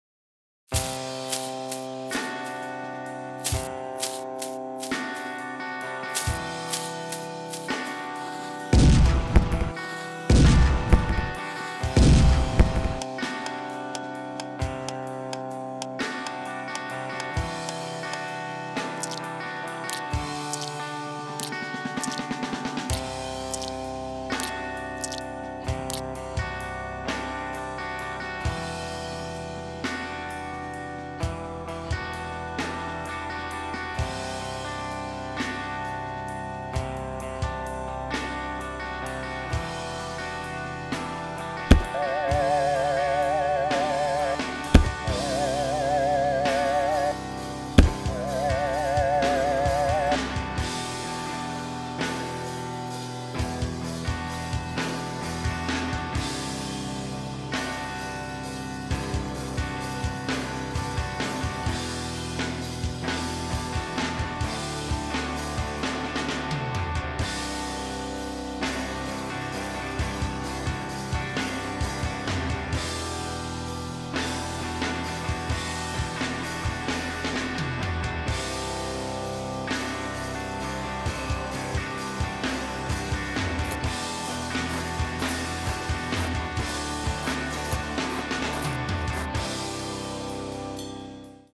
2D Animation Demo Reel
This was a demo reel I put together for a beginning aimation drawing class. This has examples of bouncing balls, pendulums, walk across, walk cycle, moprhs, etc.